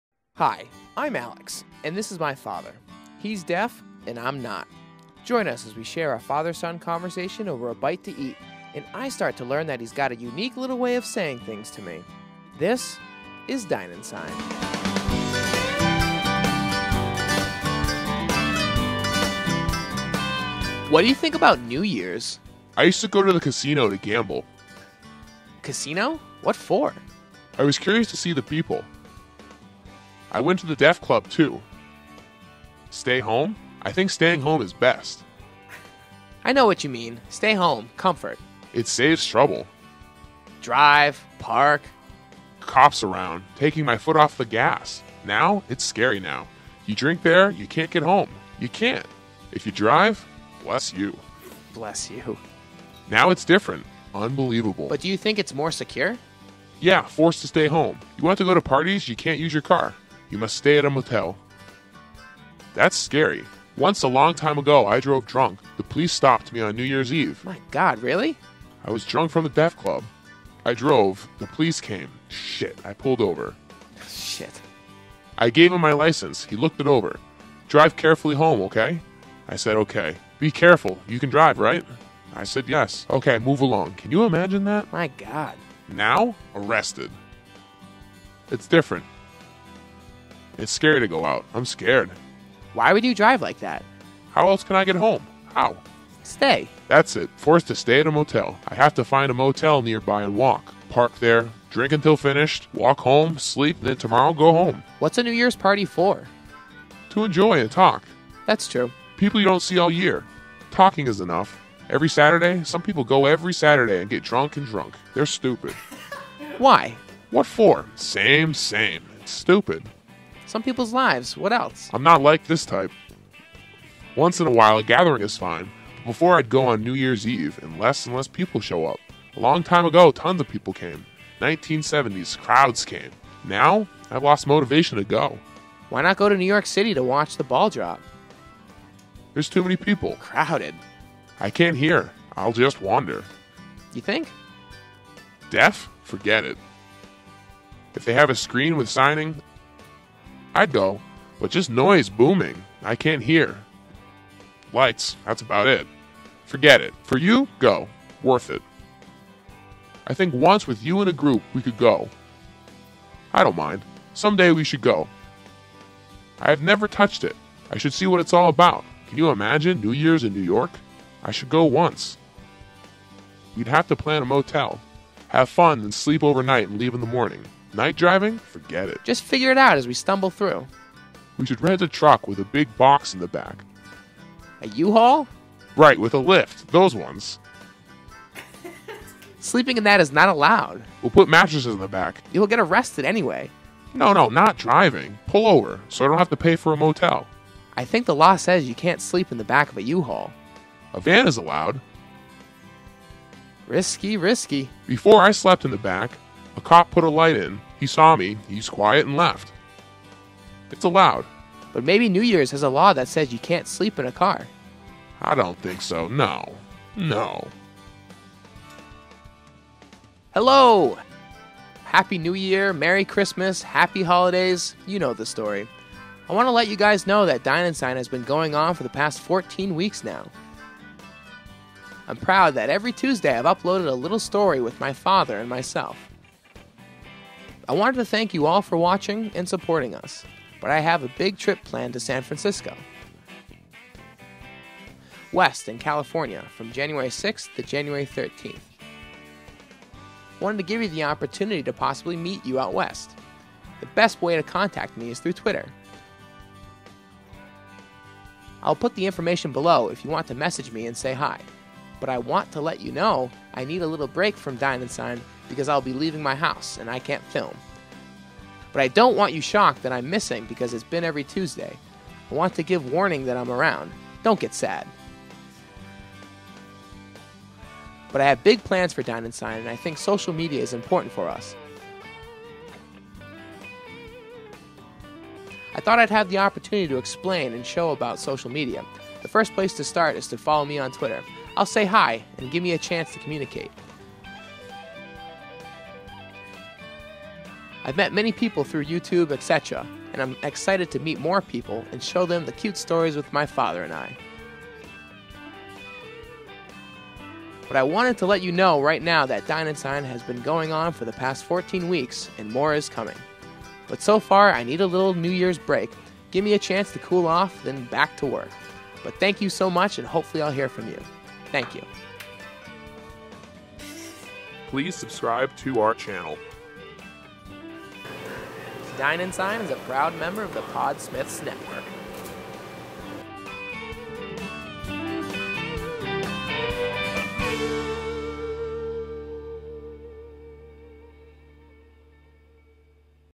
New Years Driving? Dine & Sign Episode 15
New Years and drinking.. a terrible mix? Perhaps a UHAUL is in order.
Big plans for Dine & Sign and my word of my Trip to San Francisco.

Dine & Sign is the video documentation between a deaf father and his hearing
son. I am beginning to understand where all my interests and upbringing comes from yet still have many more questions for my father. I love my dad and love his thoughts even more. I felt it was time to share with the world. Join us for a bite to eat.

Animated Intro by: Andrew Gallagher.

Podsmiths Production 2011